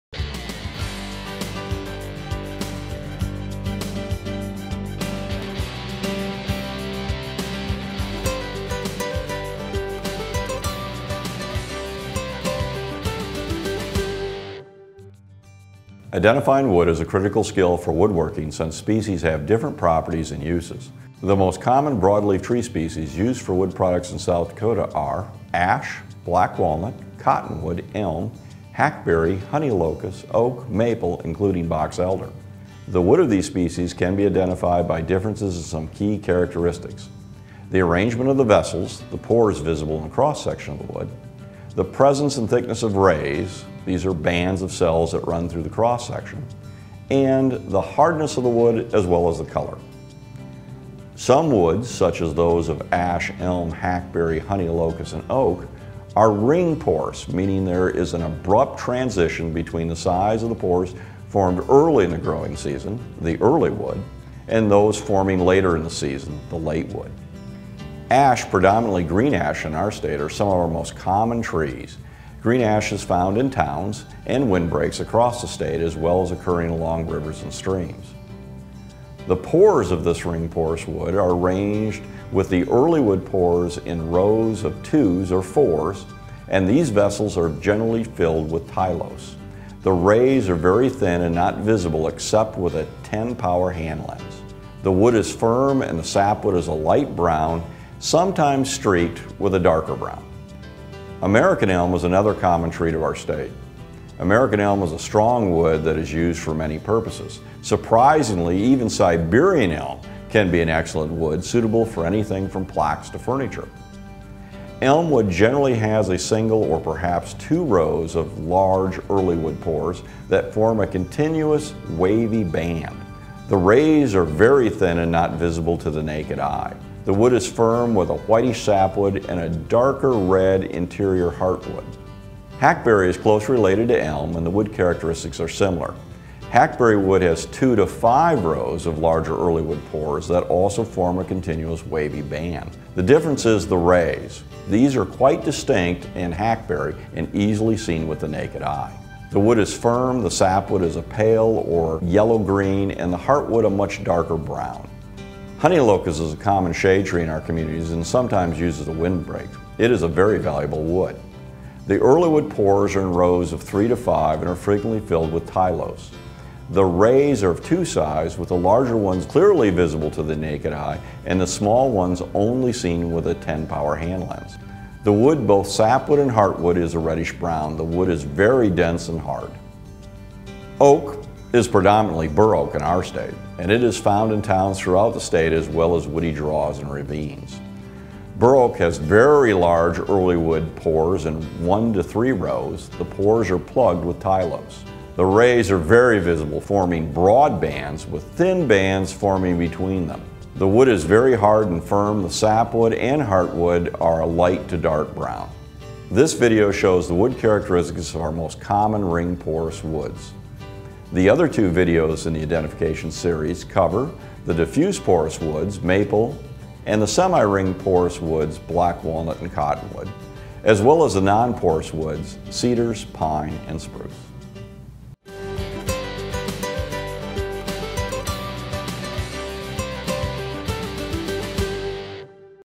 Tree Species for Wood: Ring Porous
Identifying wood is a critical skill for woodworking since species have different properties and uses. Watch this, as well as, the other iGrow videos in the Urban Logging series for more information.

Comments by: John Ball, SDSU Extension Forestry Specialist